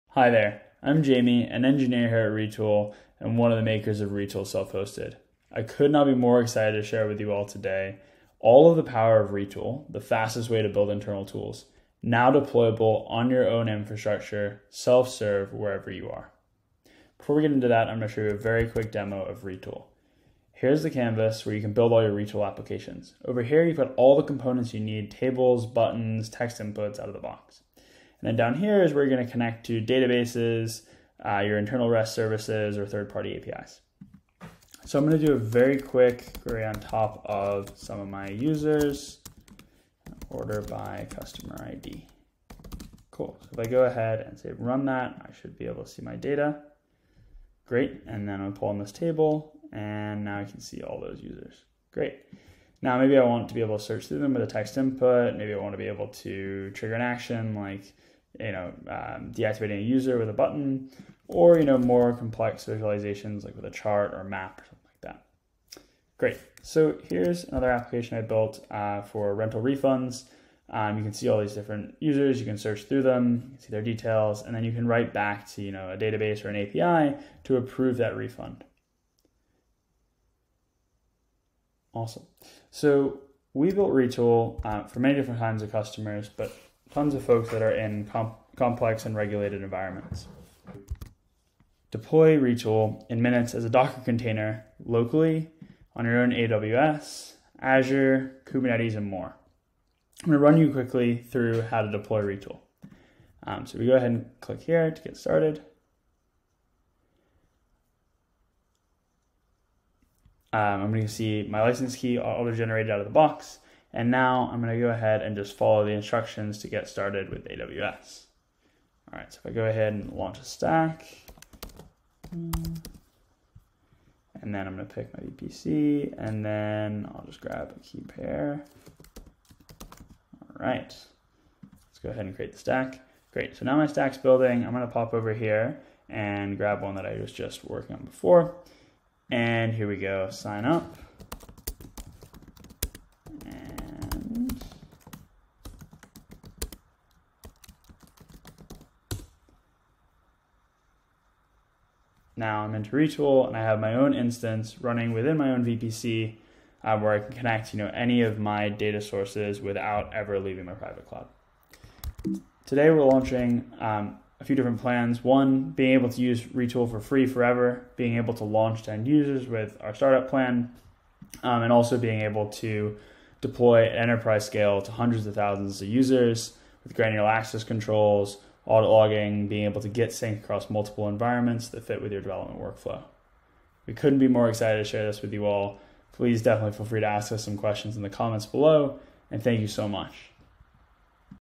Retool, self-hosted
Our low code app builder combines the ease of drag-and-drop visual components with APIs, logic, and escape hatches so developers can custom code whatever they need. Now with self-serve plans that you can deploy on your own private network.